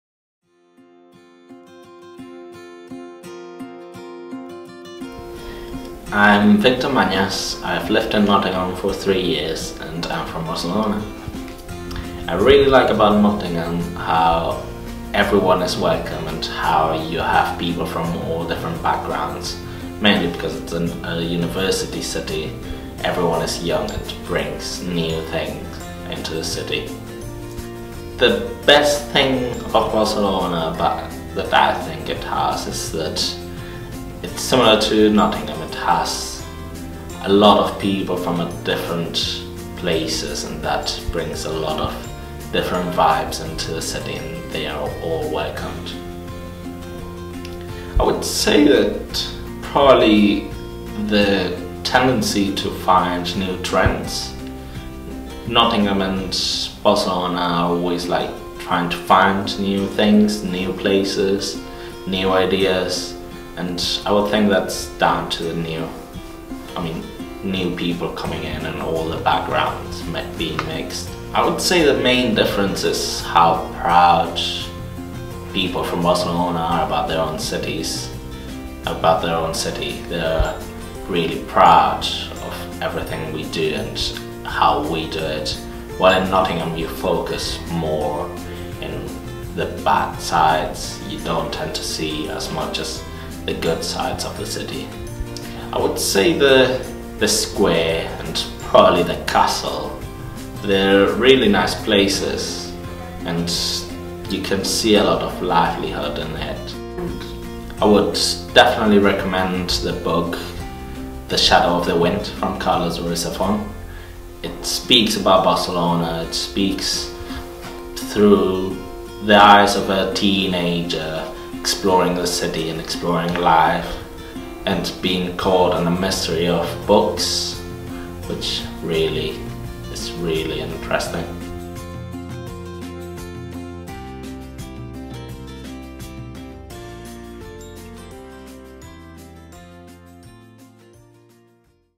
Barcelona in Nottingham | UNESCO Sister Cities
We meet Victor, a guy from Barcelona who now lives in Nottingham, and asked him how the two literary cities compare.